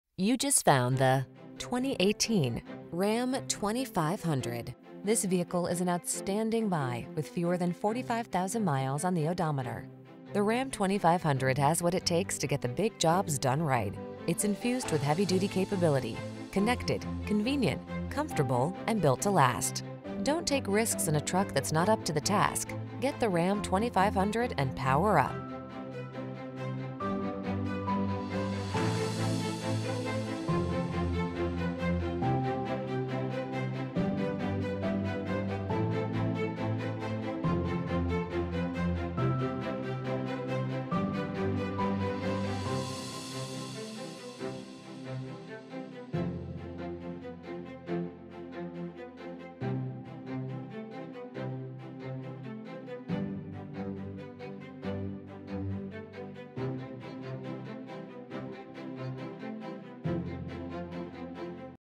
2018 Ram 2500 Power Wagon 4x4 Crew Cab 64 Box Dartmouth, New Bedford, Raynham, Westport, Fall R...
Pre-Owned 2018 Ram 2500 Power Wagon 4x4 Crew Cab 64 Box available at Colonial South Chevrolet located in 361 State Rd, North Dartmouth, Massachusetts, 02747. Servicing the Dartmouth, New Bedford, Raynham, Westport, Fall River area.

You just found the  2018  Ram 2500.  The Ram 2500 has what it takes to get the big jobs done right. It's infused with heavy-duty capability, connected, convenient, comfortable, and built to last. Don't take risks in a truck that's not up to the task. Get the Ram 2500 and power up.